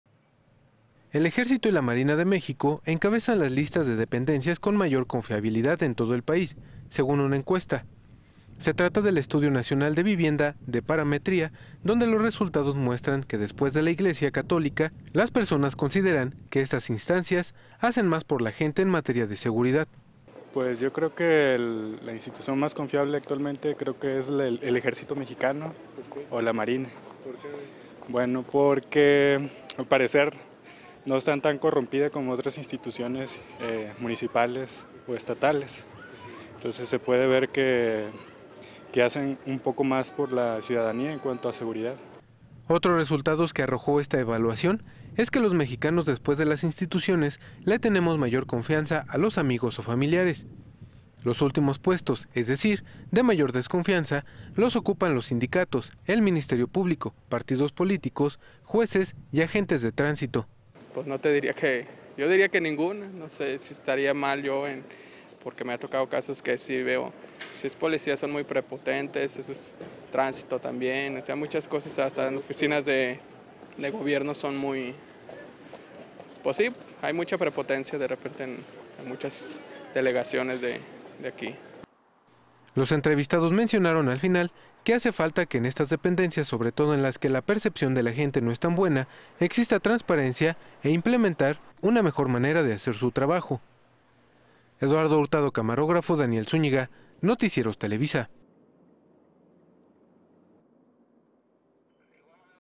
Bajio Noticias - ¿En quién confiamos?
Sabe usted, ¿Cuáles son las instituciones más confiables actualmente en nuestro país?